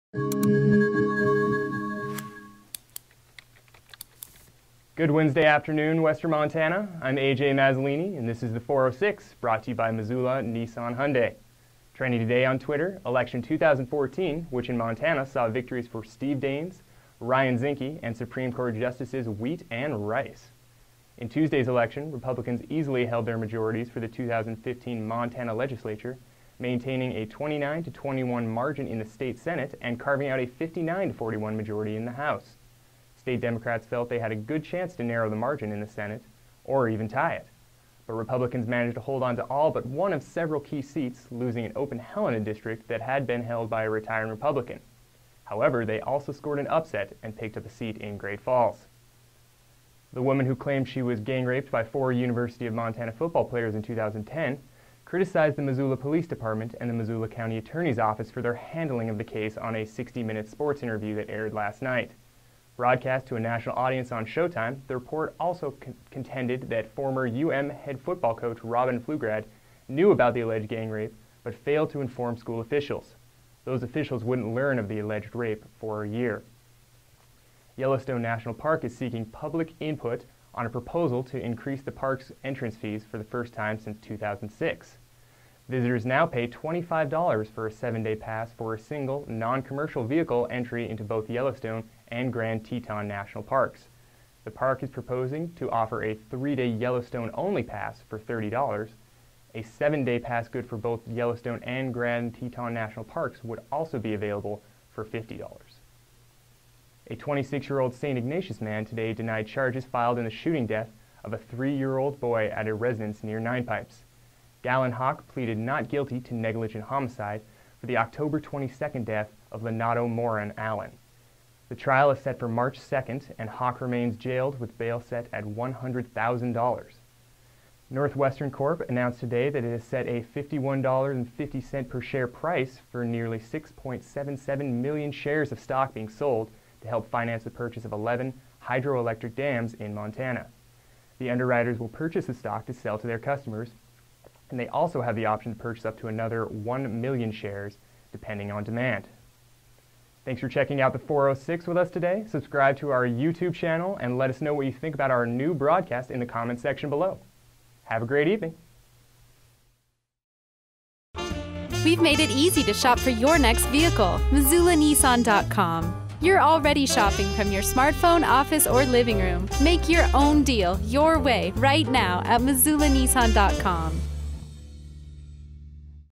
The 4:06 for Wednesday, November 5, 2014
Missoulian reporter AJ Mazzolini presents the latest news you need to know about today's headlines in about three minutes.